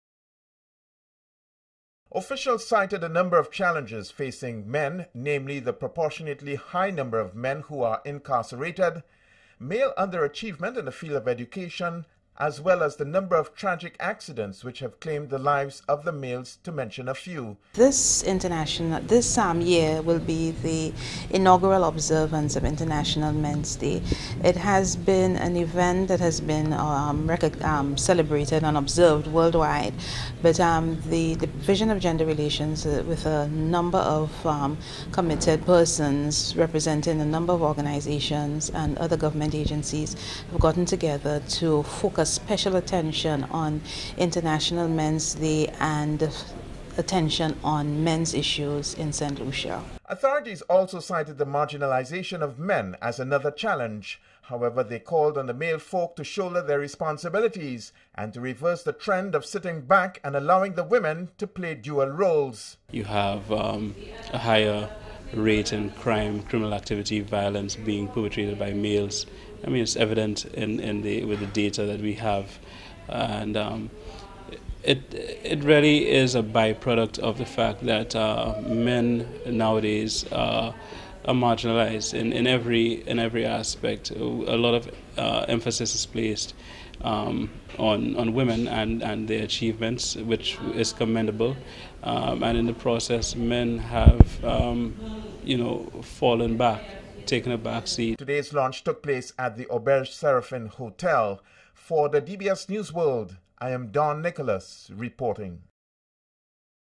Boys to men
The Division of Gender Relations is preparing to observe  International Men's Day slated for November 19th.